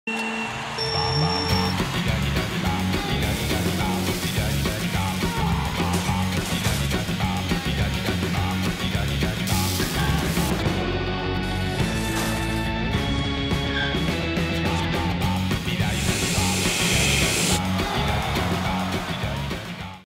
ROWENTA RH92 (épisode 1) - Aspirateur balai - Vidéo produit Vandenborre.be
Vidéo produit ROWENTA RH92 - Aspirateur balai - disponible sur Vandenborre.be